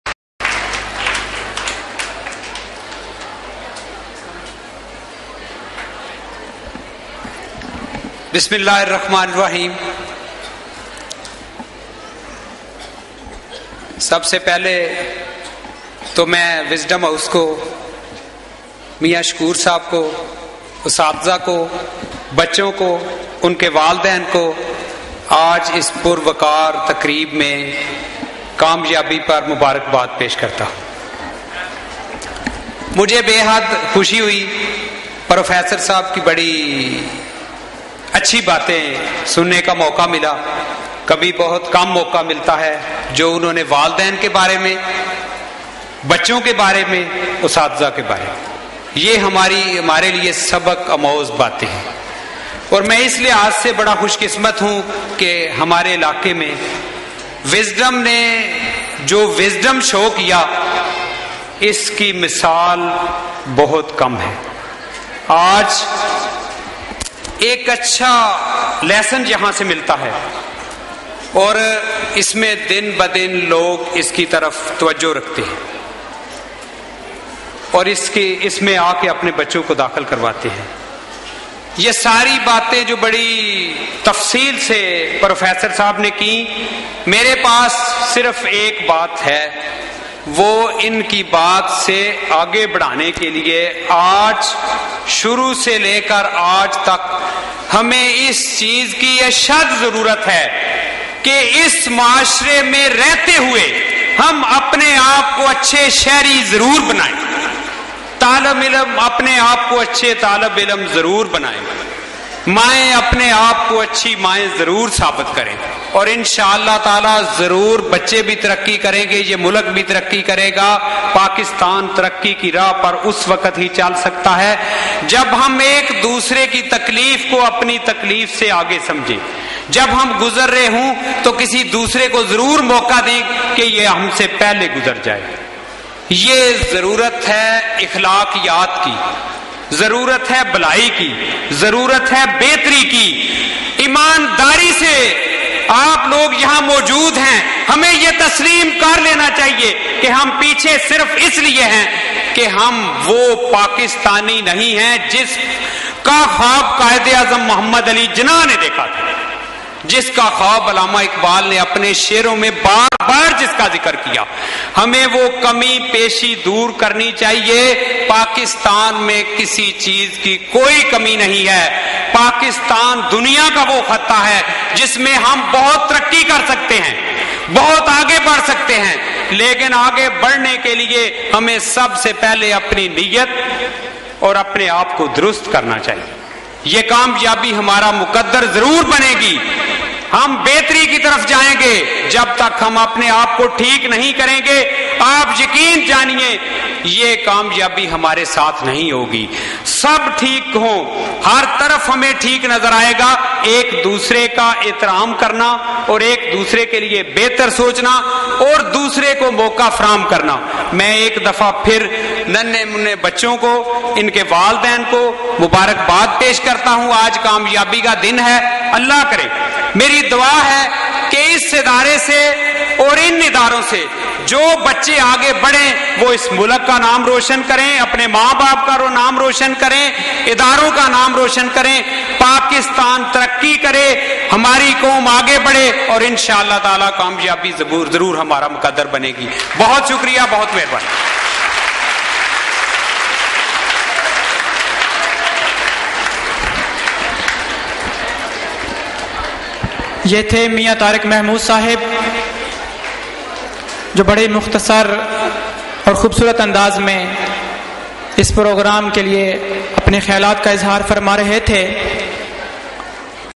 Mian Tariq MPA addressing.avi
Annual Result Day 2012, Mian Tariq Mehmood, MPA